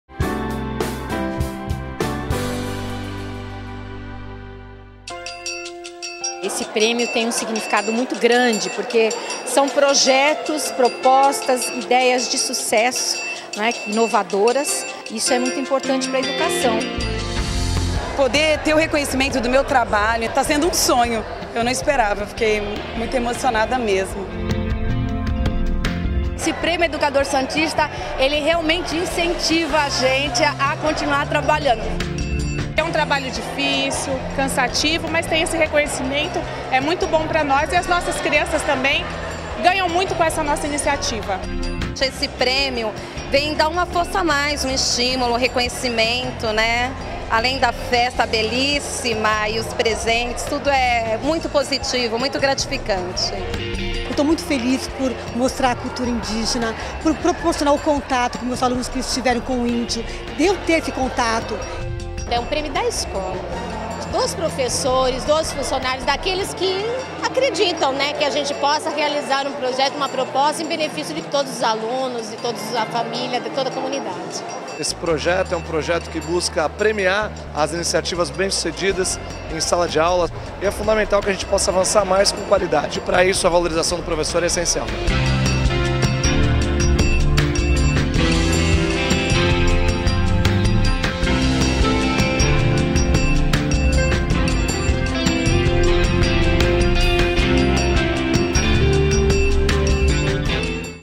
Prêmio Educador Santista: ideias inovadoras merecem reconhecimento
Confira imagens da festa de premiação e depoimentos das vencedoras desta iniciativa que valoriza ideias e ações de sucesso na sala de aula e em prol da formação de cidadãos.